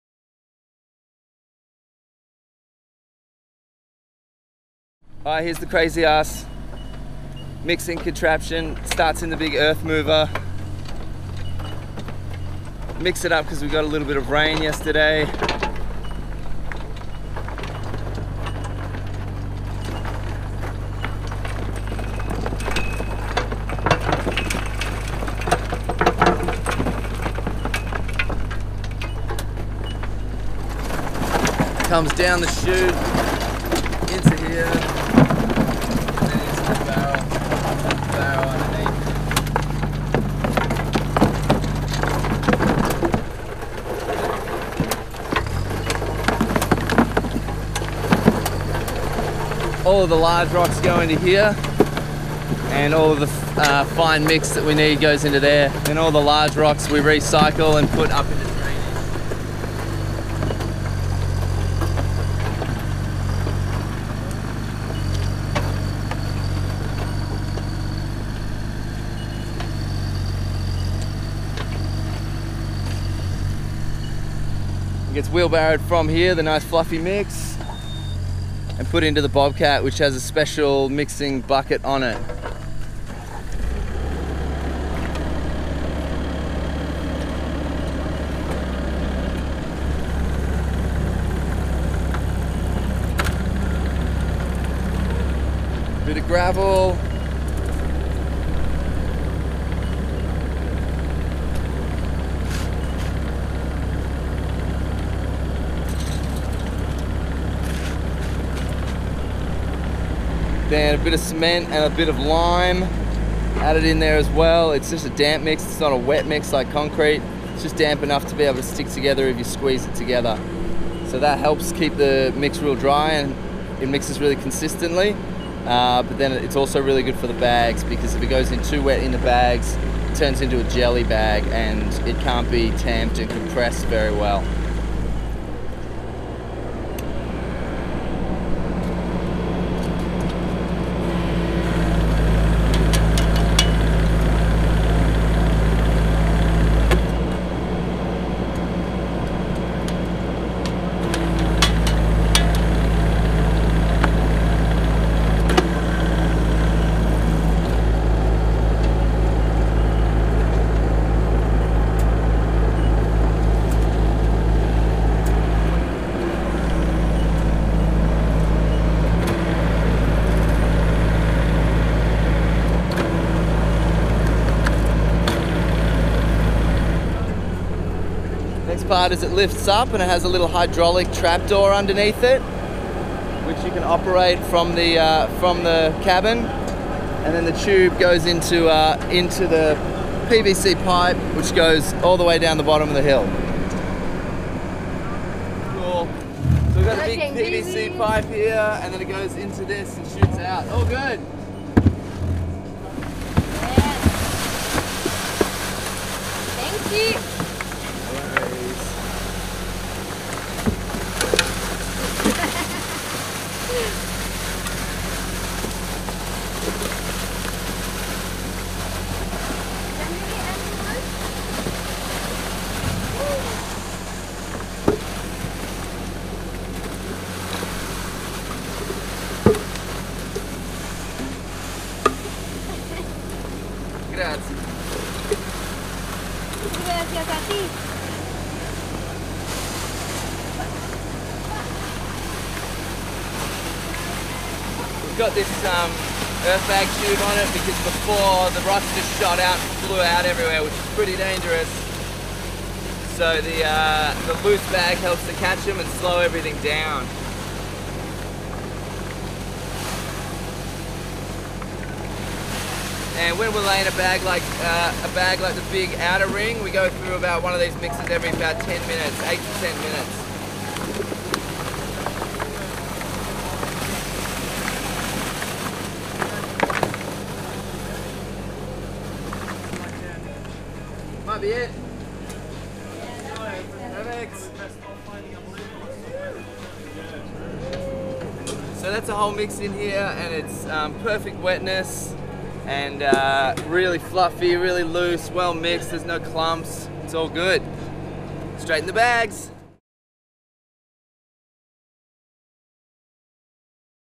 Soil Mixing Machine
This is a run-through of the soil mixing and sifting machine built by Will Eastlake of Integrated Biotecture Design at The Green House at Narara Ecovillage NSW.

We're using the mix to fill SuperAdobe Bags to build the underground Hobbit Studio, which is part 1 of an ambitious natural building.